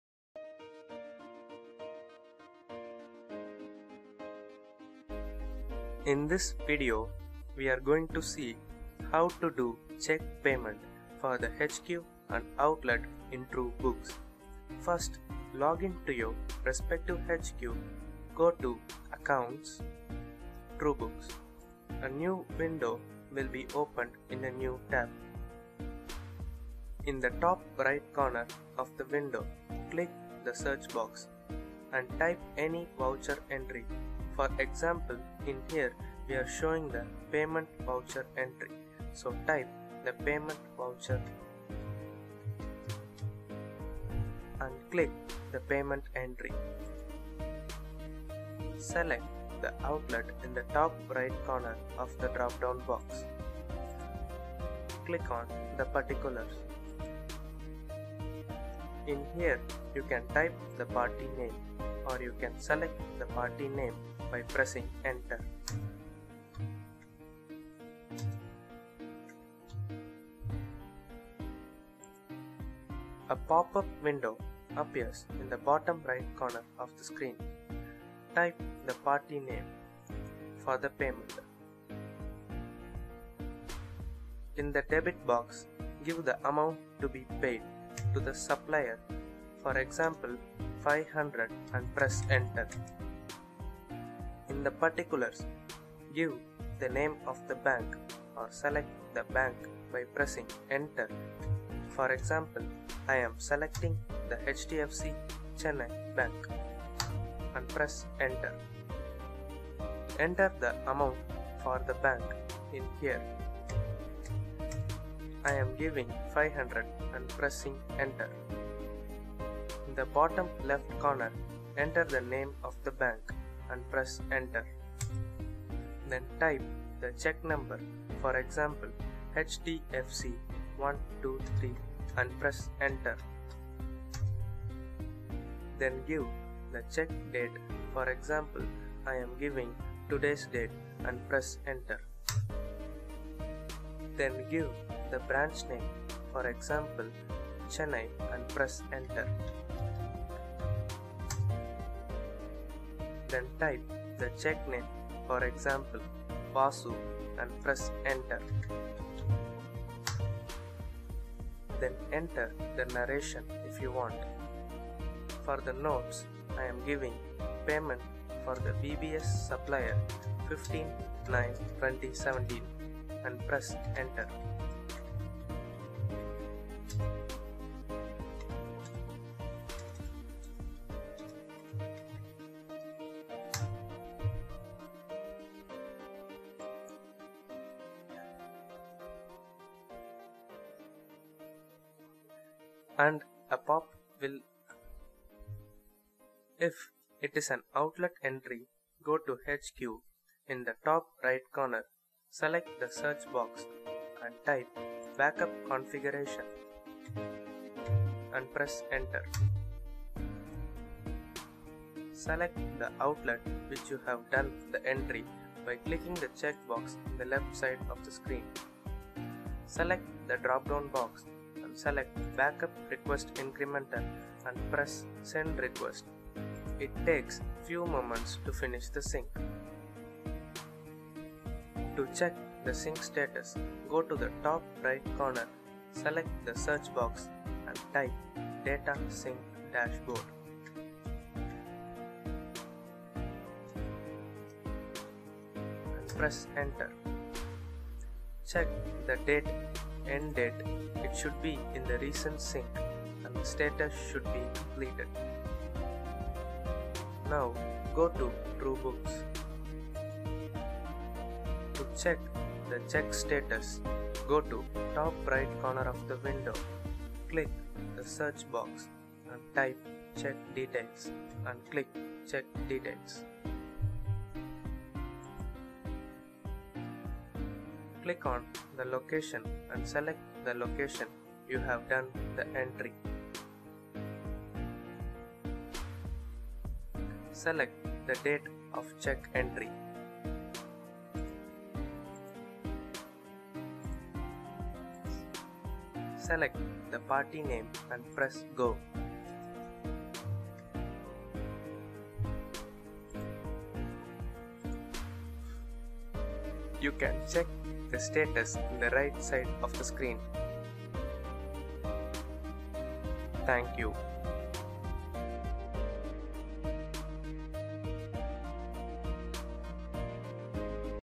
cheque entry in Truebooks Tutorial
In this Video we are seeing how to make an cheque entry for the HQ and Outlet in Truebooks